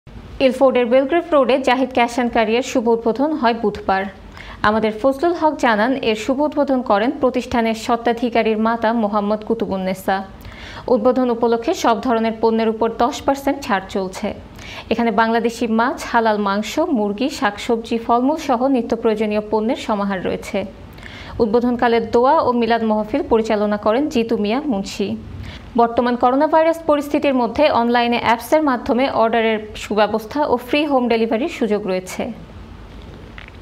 ইলফোর্ডের বেল্গ্রেভ রোডে জাহিদ ক্যাশ অ্যান্ড ক্যারি এর শুভ উদ্বোধন হয় বুধবার।
ইলফোর্ডের বেল্গ্রেভ রোডে জাহিদ ক্যাশ অ্যান্ড ক্যারি এর শুভ উদ্বোধন হয় বুধবার।  আমাদের ফজলুল হক  জানান, এর শুভ উদ্বোধন করেন প্রতিষ্ঠানের স্বত্বাধিকারির মাতা মোসাম্মাদ কুতুবুন নেসা।  উদ্বোধন উপলক্ষে  সব ধরনের পণ্যের উপর 10% ছাড় চলছে।  এখানে বাংলাদেশি মাছ, হালাল মাংস, মুরগি, শাকসবজি ফলমূল সহ নিত্যপ্রয়োজনীয়  পণ্যের সমাহার রয়েছে।  উদ্বোধন কালে দোয়া ও মিলাদ মাহফিল পরিচালনা করেন জিতু মিয়া মুন্সি। বর্তমান করোনাভাইরাস পরিস্থিতির মধ্যে অনলাইনে অ্যাপসের মাধ্যমে অর্ডার এর সুব্যবস্থা ও ফ্রি হোম ডেলিভারির সুযোগ রয়েছে।
iON TV
SKY 782